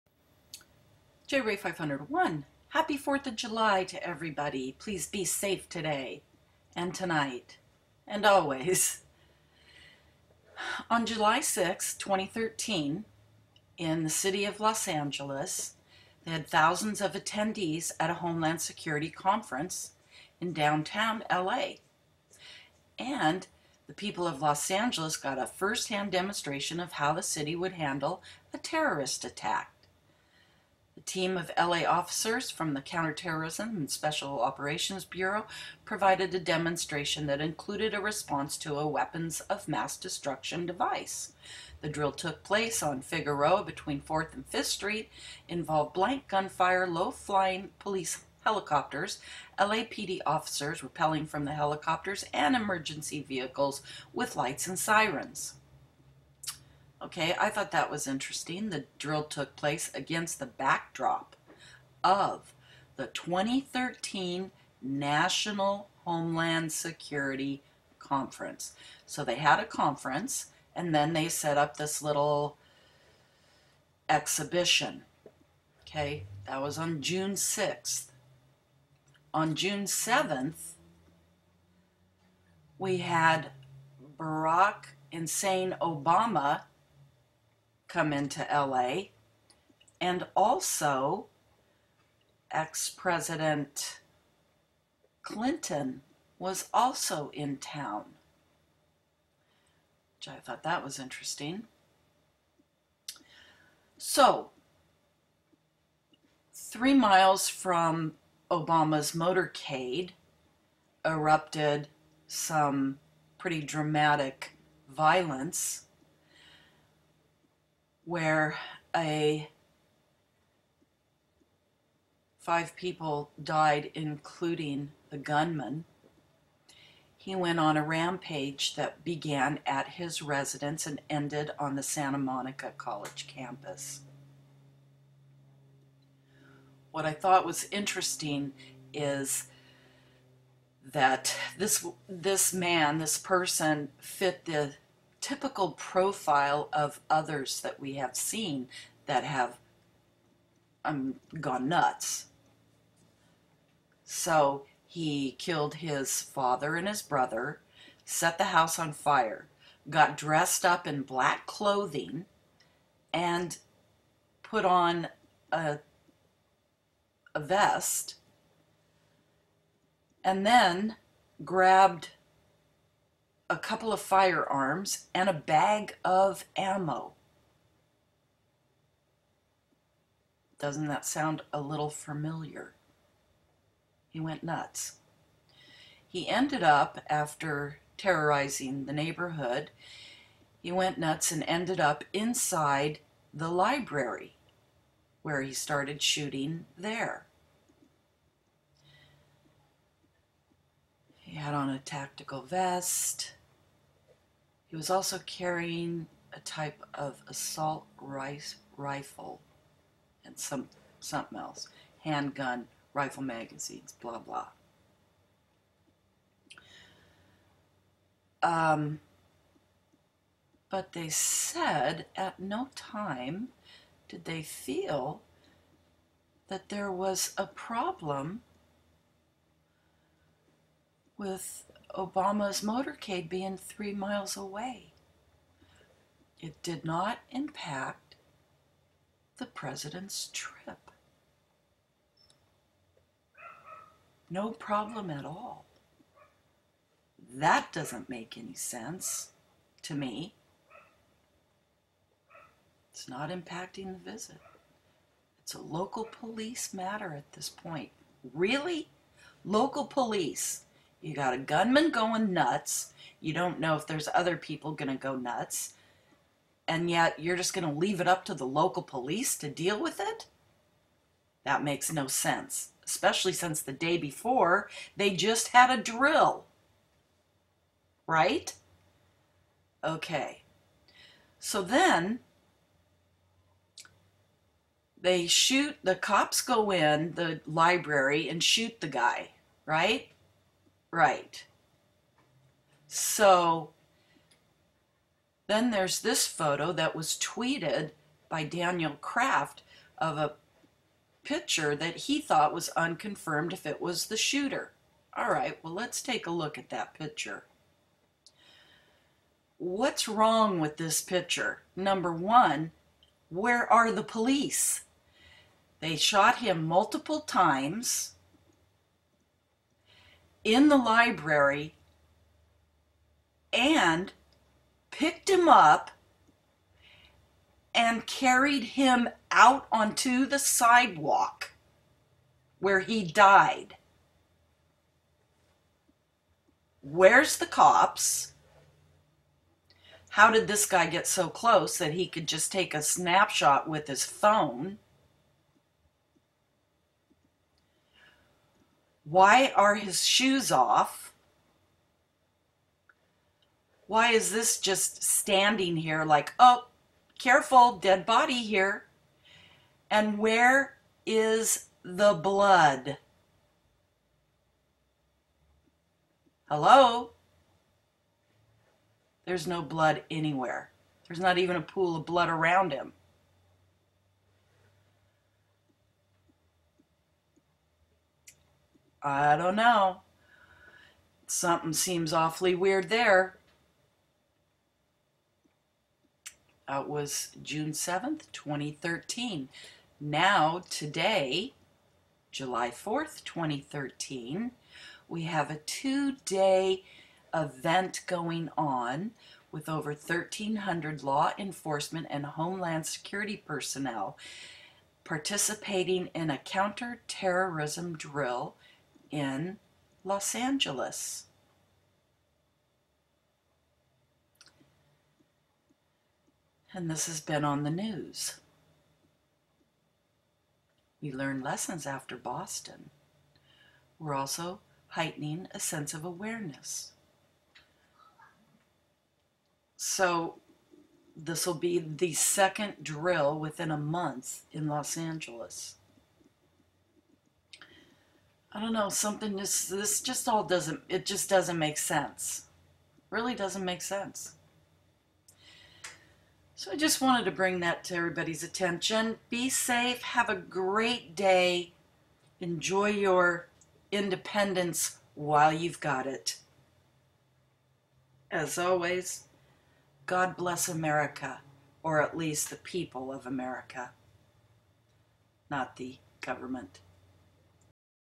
2 SPECIAL OPS DRILLS IN 1 MONTH in Los Angeles, California
June 6, 2013 LOS ANGELES  — Thousands of attendees at a Homeland Security conference in downtown Los Angeles Thursday got a first-hand demonstration of how the city would handle a terrorist attack.
A team of LAPD officers from the Counter-Terrorism and Special Operations Bureau provided a demonstration that included a "response to a weapons of mass destruction device." The drill took place against the backdrop of the 2013 National Homeland Security Conference.

June 7, 2013 President Obomb-ah was in L.A. at a Democratic Party fundraiser. He was scheduled to deliver remarks around 12:30 p.m. At the exact same time a lone gunman went on a rampage. The crime began on the 2000 block of Yorkshire Avenue, where two bodies were found in a burning house. After the shooter left the house, he shot at one vehicle, seriously injuring the driver. He then carjacked another driver, forcing her to drive him. He ended up inside the library at Santa Monica College, the shooter engaged in a gunfight with police and sustained at least one gunshot wound. He was carried out of the building by authorities and later died of his wounds on the sidewalk.
The Secret Service says the President's trip was not impacted by the shooting.
"We are aware of the incident and it is not impacting the visit," said Secret Service Spokesman Edwin M. Donovan at the time. "It's a local police matter at this point."

July 4, 2013 Operation Independence is a two-day, high-visibility training exercise that is "all about keeping L.A. safe," according to Nicole Nishida of the Los Angeles County Sheriff's Department. Over 1,300 law enforcement and homeland security personnel participated in a counter-terrorism drill in downtown Los Angeles. Sheriff's deputies -- accompanied by explosive-sniffing dogs -- will have a more visible presence at Union Station to perform random bag inspections, according to Nishida.